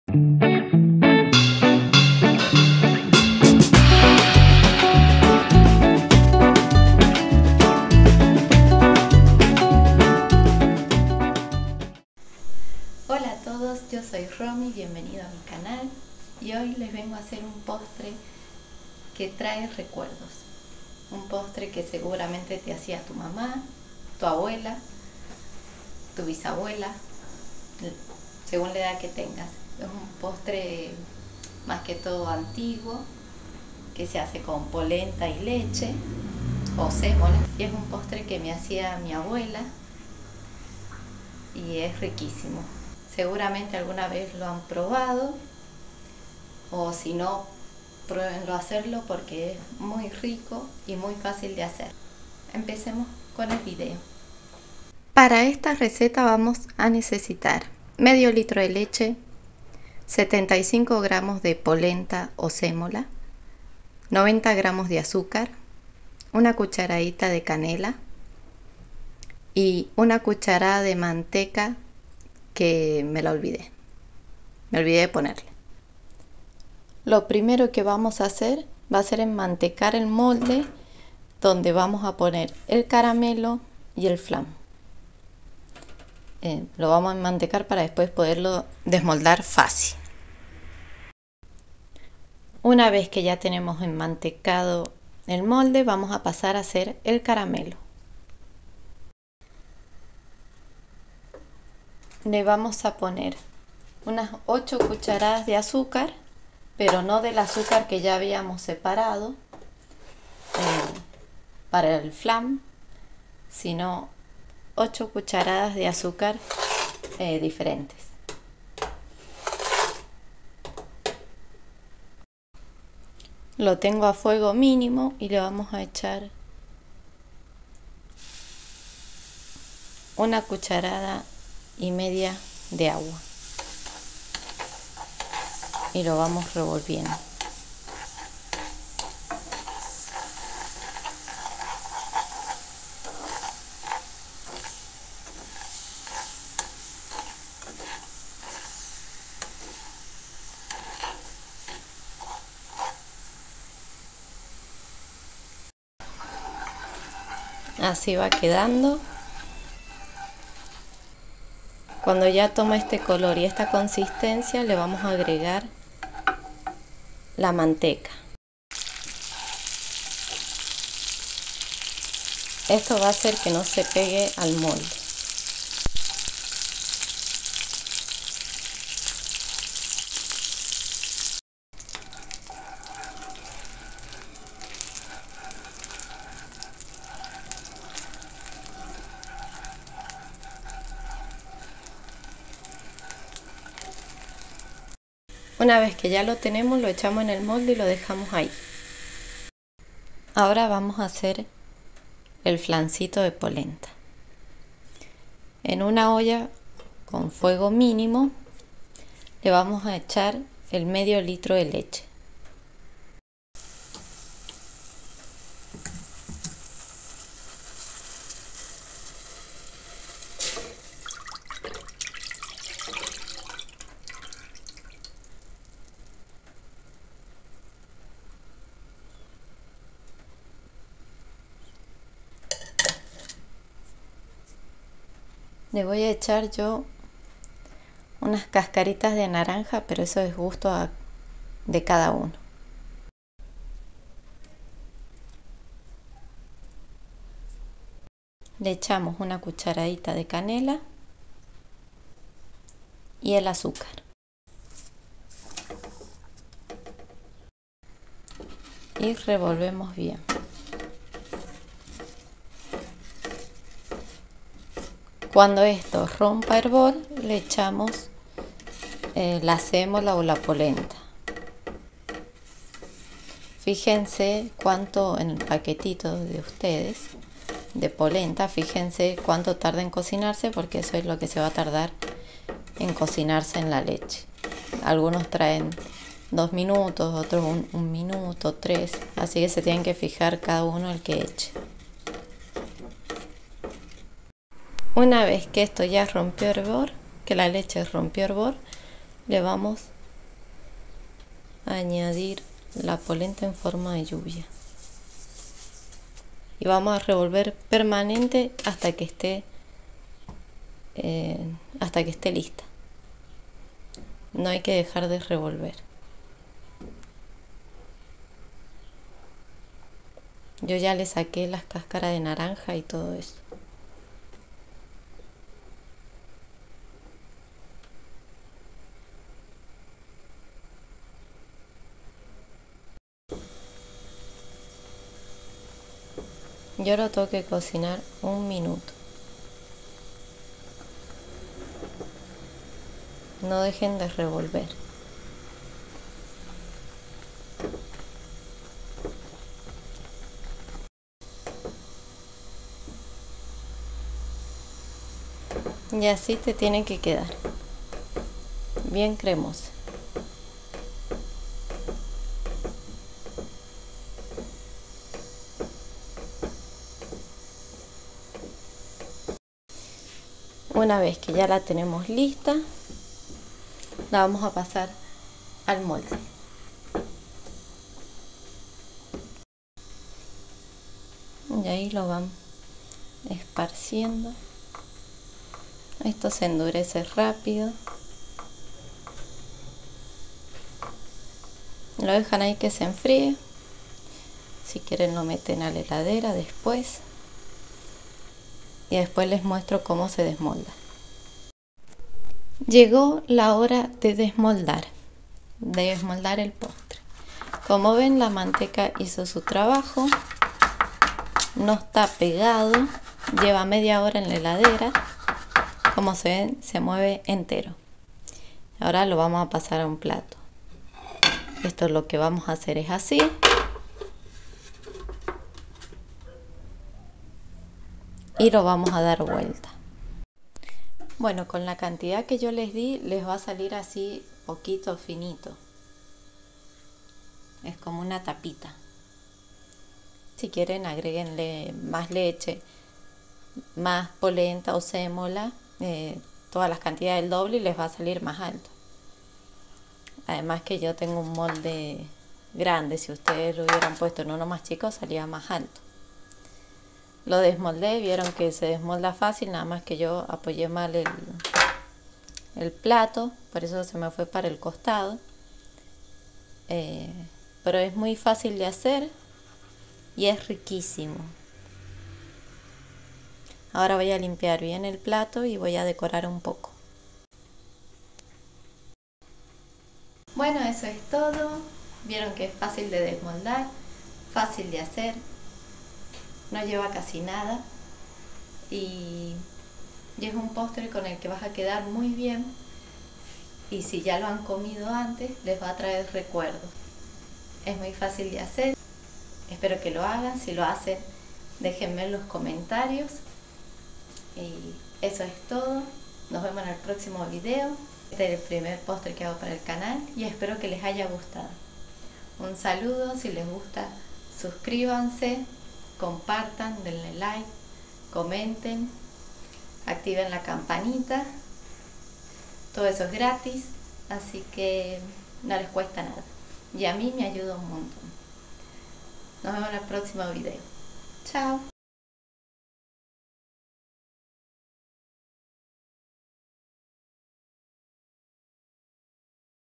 Polenta con leche/ sémola con leche/ flan de polenta/ flan de sémola/ postre Polenta/ Postre sémola
Hoy les traigo un postre de recuerdos. un postre de la abuela, rico, fácil y que quedas enamorado de este postre

Ingredientes:
1/2 litro de leche
75 g de Polenta o Sémola
90g de Azúcar
1 cucharadita de Canela

Caramelo:
8 cucharadas de Azúcar
1 cucharada y media de agua
1 cucharada de Manteca